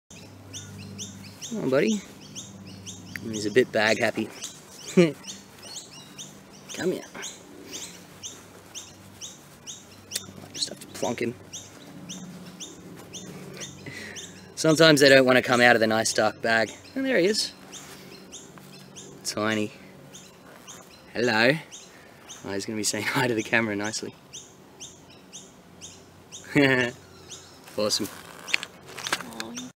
Southern Dwarf Crown Snake (Cacophis krefftii)
Watch me, Brisbane Snake Catcher Janne Torkkola, and this dwarf crowned snake.

Southern Dwarf Crowned Snake, a new species for for me! As the 8th smallest terrestrial elapid snake in Australia (with smaller spp. found in Brachyurophis, Drysdalia, Neelaps and Simoselaps), this cute little guy only reaches 345mm in total length, while our friend from today was about 250mm